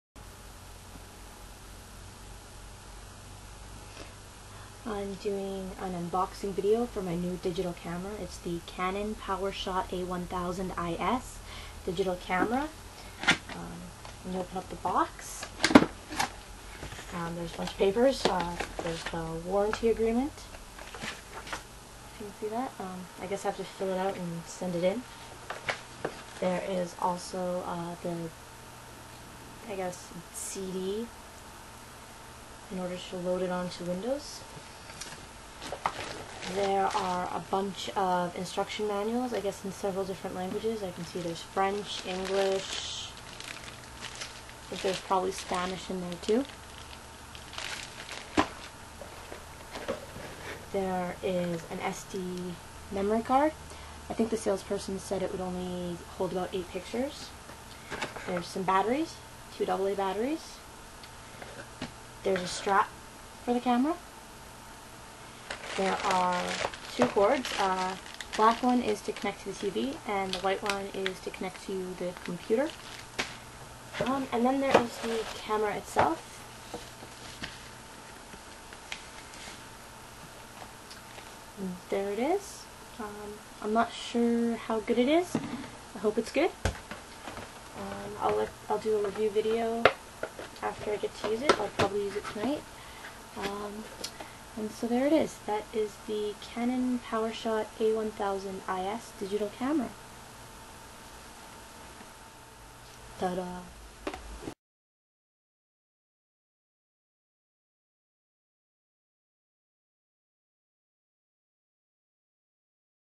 Digital Camera Unboxing
Unboxing of my new digital camera (Canon PowerShot A1000 IS).